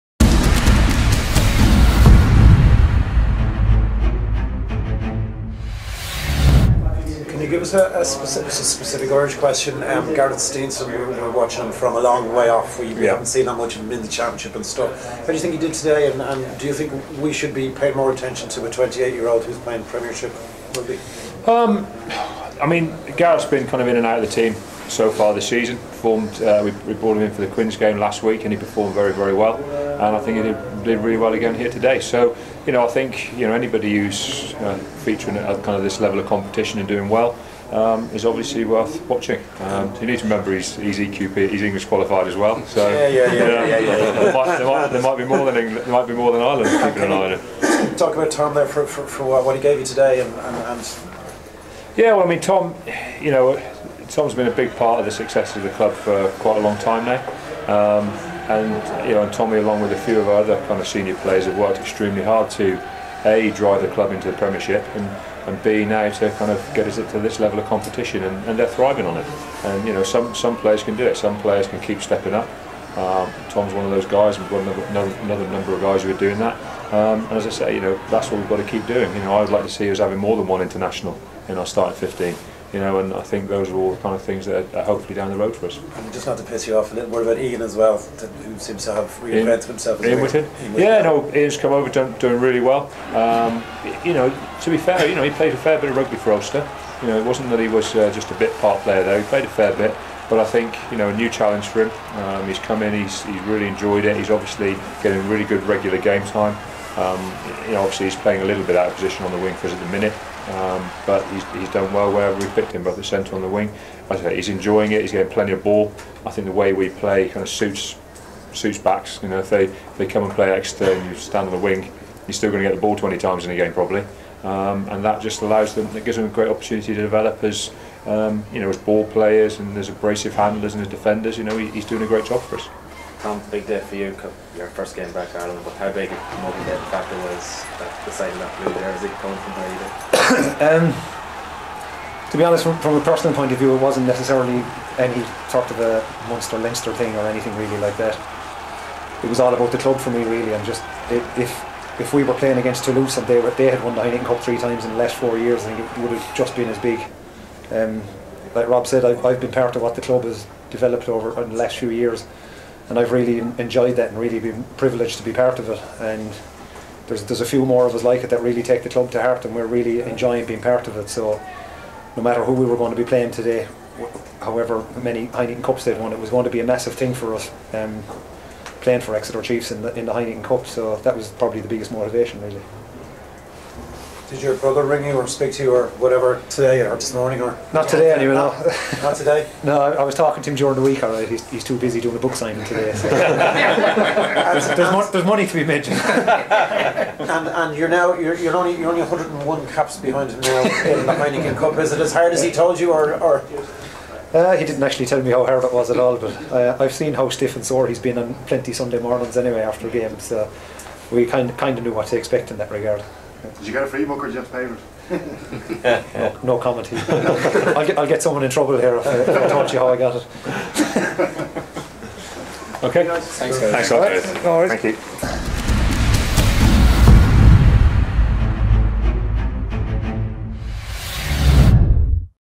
Chiefs TV Baxter & Hayes post Leinster part 2
Chiefs TV - Part 2 of the Rob Baxter & Tom Hayes Press call following Leinster vs Exeter Chiefs...